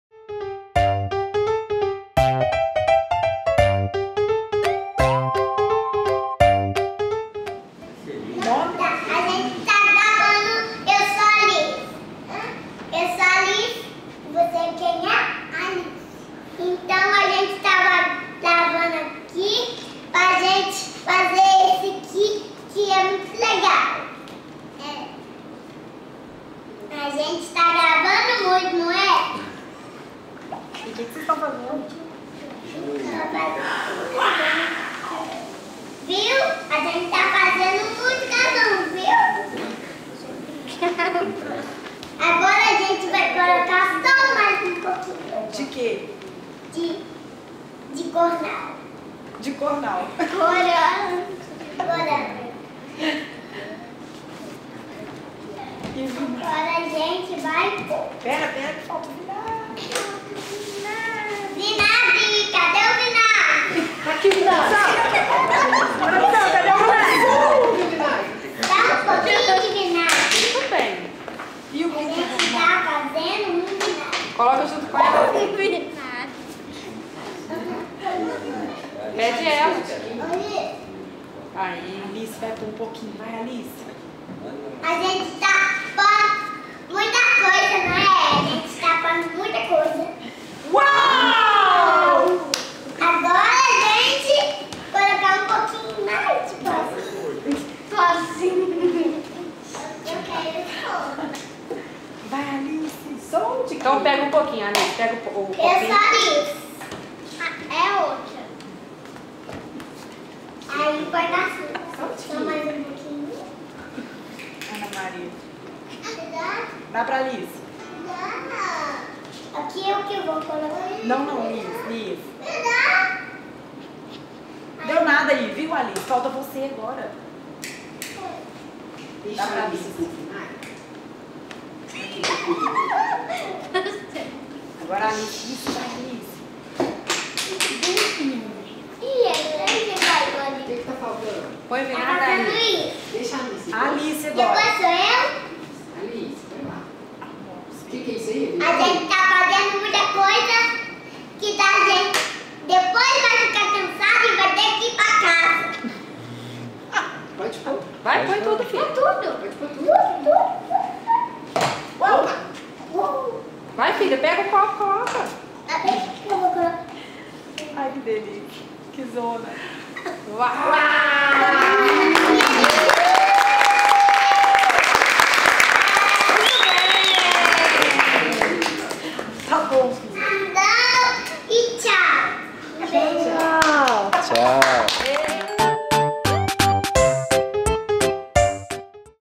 Alquimistas no Museu 2019 - 03 - 31/01/2019
A oficina Alquimistas no Museu consiste em uma serie de atividades realizadas em um laboratório montado no Ateliê Científico (espaço permanente e aberto para diálogos sobre ciência, tecnologia, sociedade e arte). Neste espaço, os visitantes aprenderam e executaram experiências, de forma lúdica e divertida. Tais experiências foram filmadas no formato web série e transmitidas posteriormente para o Youtube. Para tanto, os participantes receberam auxílio dos educadores e das educadoras para a montagem dos experimentos; dialogaram sobre os conceitos científicos referentes a cada um deles e realizaram a elaboração dos roteiros da apresentação dos experimentos.